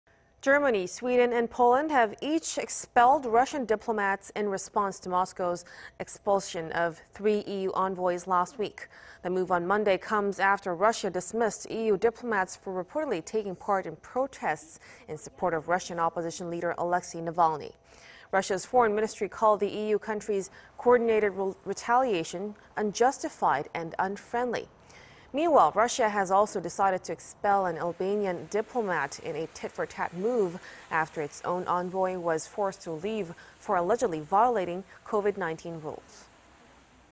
Germany, Sweden, and Poland expel Russian diplomats in response to Moscow's expulsion of EU envoys
독일•스웨덴•폴란드, 러시아 외교관 맞추방...러, 알바니아 외교관 맞추방

Germany, Sweden, and Poland have each expelled Russian diplomats in response to Moscow's expulsion of three EU envoys last week.
The move on Monday comes after Russia dismissed EU diplomats for reportedly taking part in protests in support of Russian opposition leader, Alexei Navalny.
Russia's foreign ministry called the EU countries' coordinated retaliation "unjustified and unfriendly".
Meanwhile, Russia has also decided to expel an Albanian diplomat in a tit-for-tat move,... after its own envoy was forced to leave for allegedly violating COVID-19 rules.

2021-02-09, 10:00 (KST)